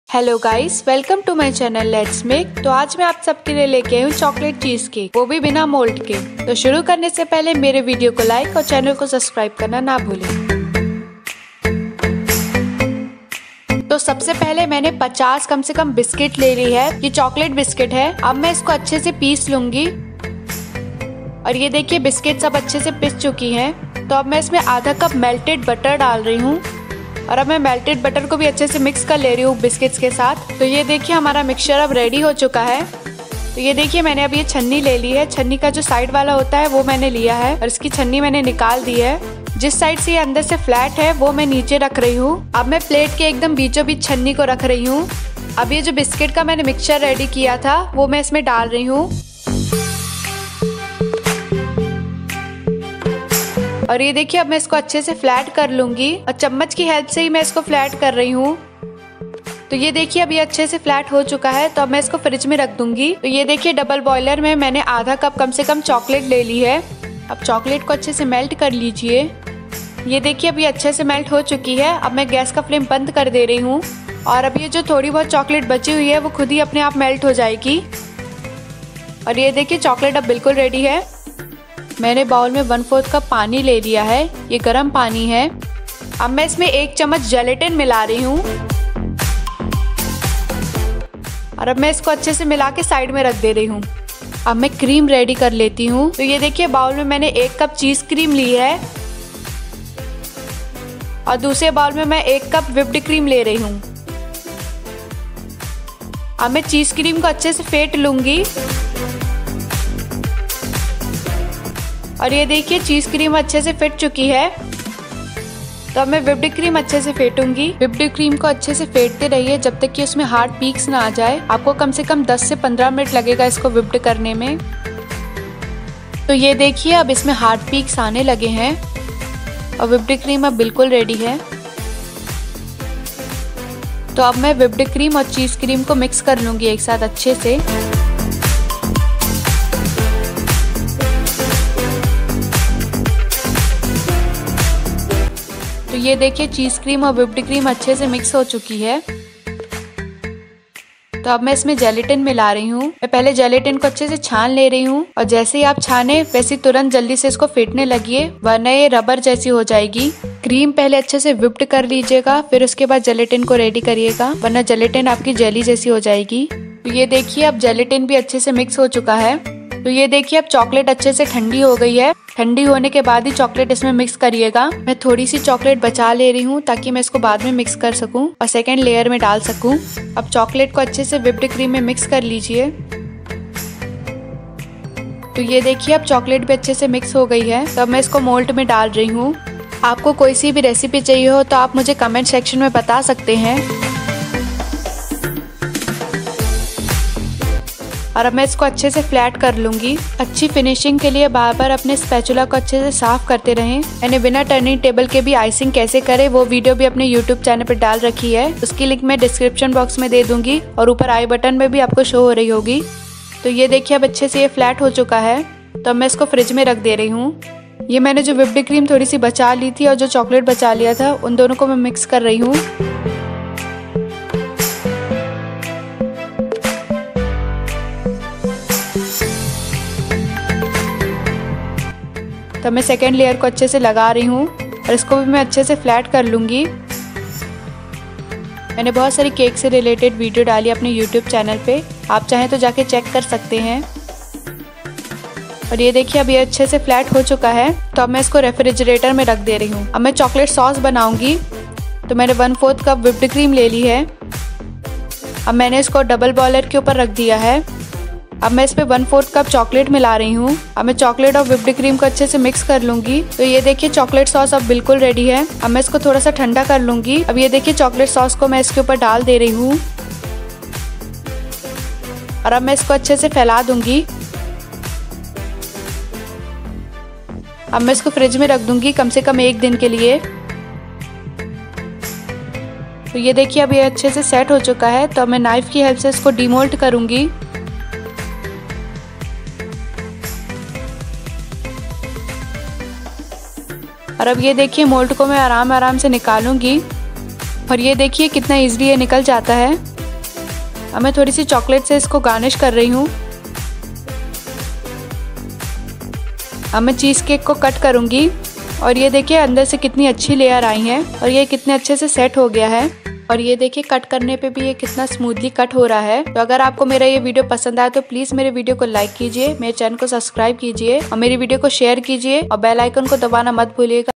CHOCOLATE CHEESECAKE WITHOUT TOOLS / GHAR PE BANAYE CHOCHOLATE CHEESECAKE BINA KISI MOLD K CHANNI ME
CHOCOLATE CHEESECAKE WITHOUT TOOLS/ GHAR PE BANAYE CHOCHOLATE CHEESECAKE BINA KISI MOLD KE CHANNI ME

INGREDIENTS

     FOR  BASE
50 - BISCUITS
1/2Cup - BUTTER

      FOR  CREAM
1Cup - CHEESE CREAM
1Cup - WHIPPED CREAM
1/4Cup - WARM WATER
1tbsp - GELATIN

       FOR  CHOCHOLATE SAUCE
1/4Cup - WHIPPED CREAM
1/4Cup - CHOCHOLATE